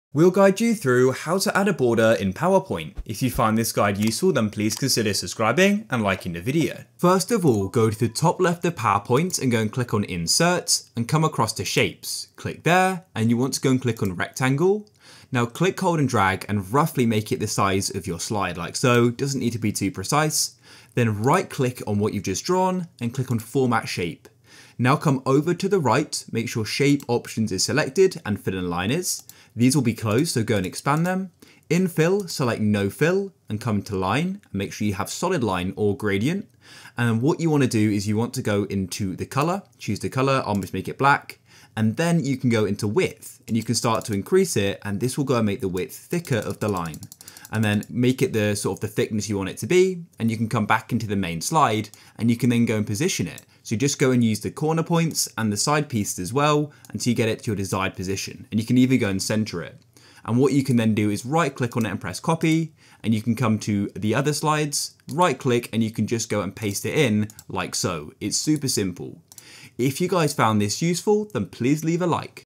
How To Add Borders In Powerpoint - Full Guide
Learn how to add borders in powerpoint in this video.

GuideRealm